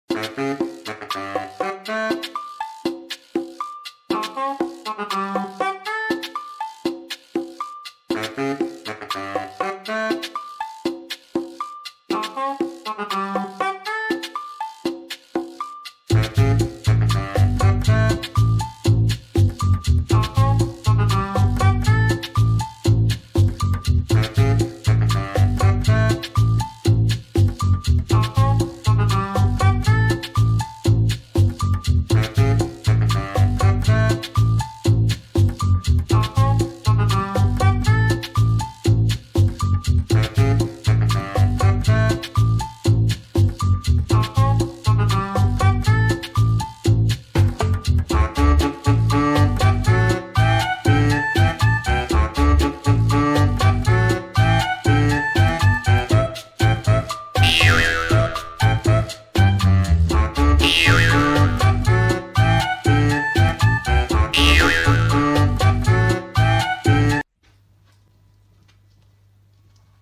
エリザベスカラーがバインバインしちゃう猫
フク姫が
まだエリザベスカラーを付けていた時に
同居人のiPhoneで
スロー撮影した映像です。

スマートにトントンと降りてくる
シシマルと対照的に
フク姫は…(;´∀｀)

悪いのはフク姫じゃない！
エリザベスカラーの形状のせいだから…！
(´ﾟдﾟ｀)